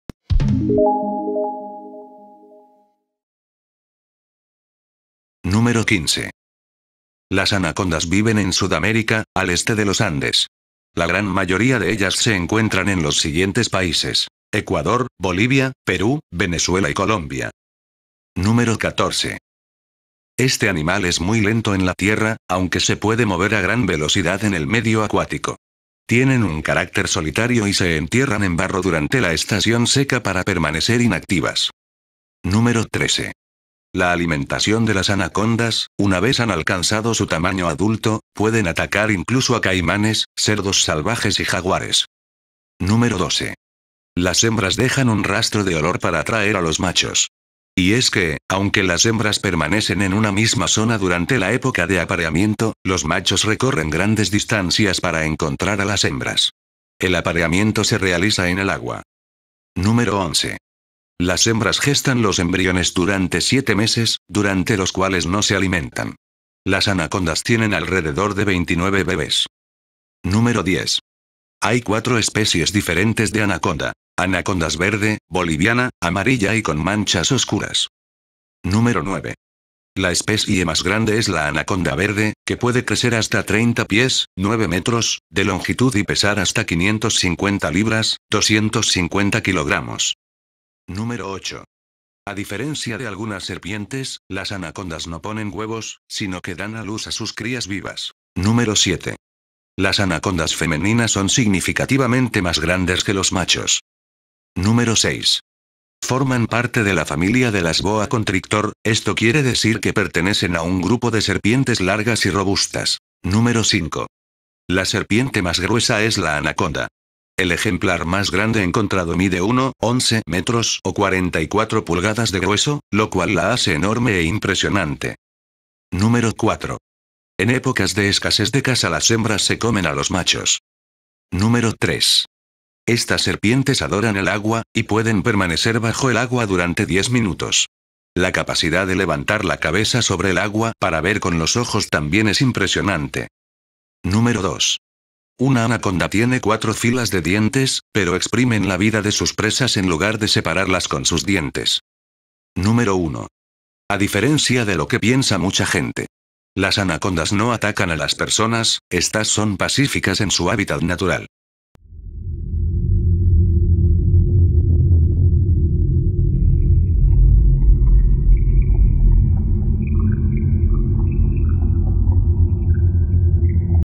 15 curiosidades sobre las anacondas
La anaconda es una especie del género Eunectes, lo que significa en griego: “buena nadadora”, una de sus características principales es que se mueve con facilidad en el agua, es semiacuática, y pasa la mayor parte de su tiempo sumergida.